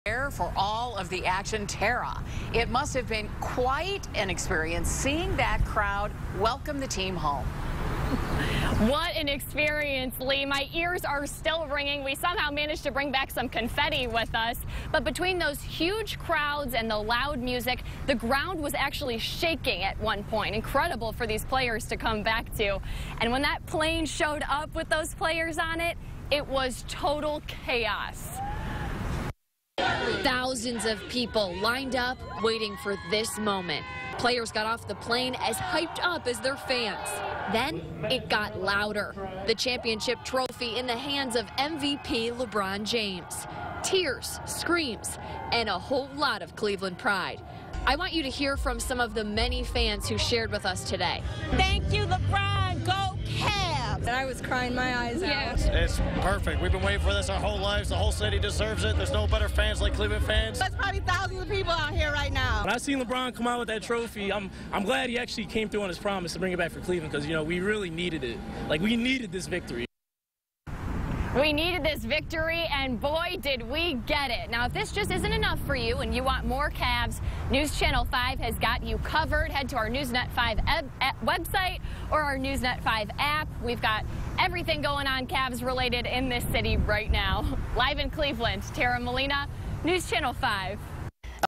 Cavs land in Cleveland, fans react- Tara Molina
Live on Five